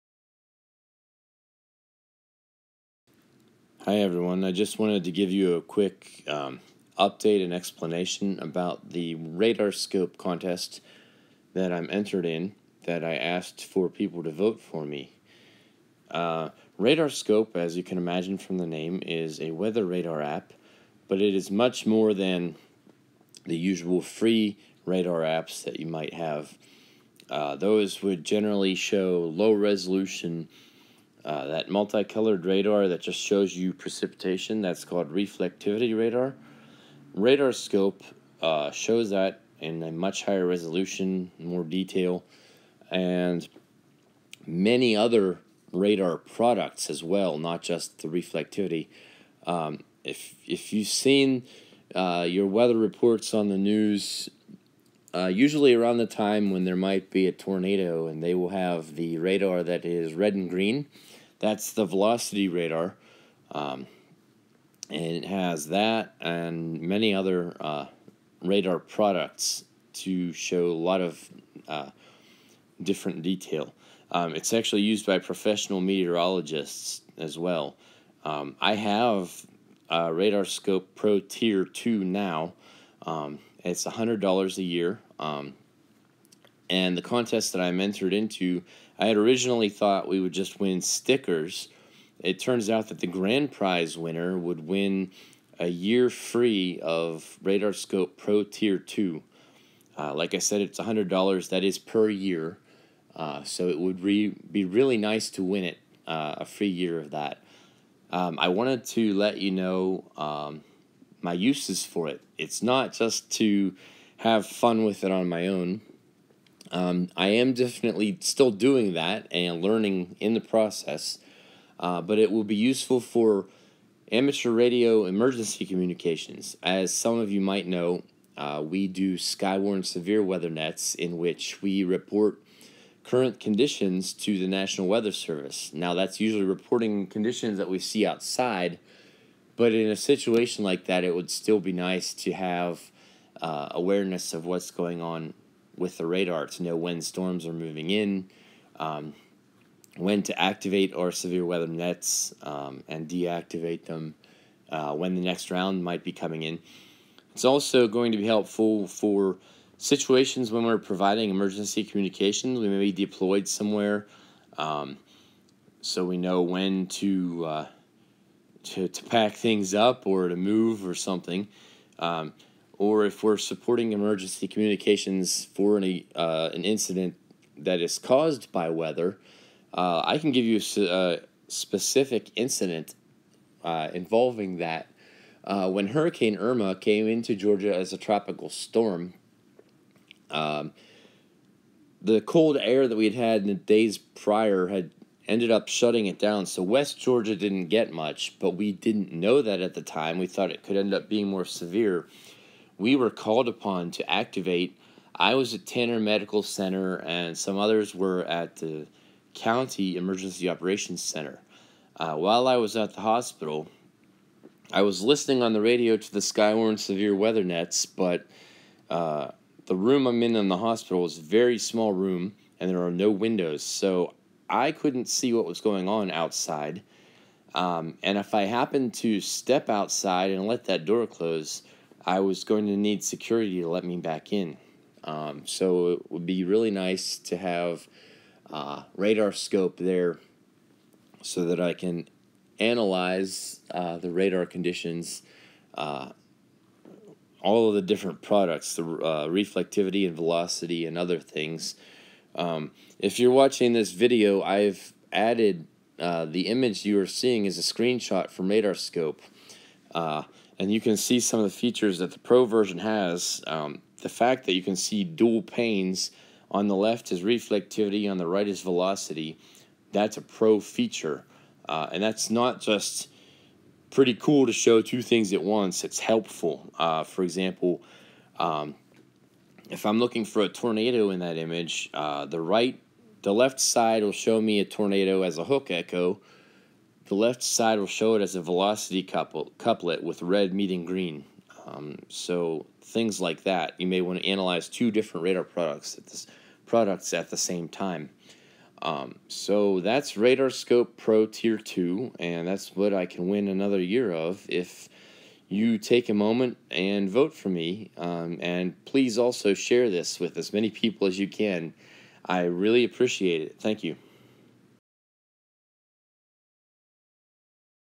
Help Me Win RadarScope Pro Tier 2!
I would appreciate your vote every day until the 6th to help me win RadarScope Pro Tier 2, an advanced radar app that is used by professional meteorologists. It will be very useful in amateur radio emergency communications. The link to vote is

And I know I said "left" twice at the end of the video when pointing out which radar was one the left and which was on the right. I put the amateur in amateur radio :)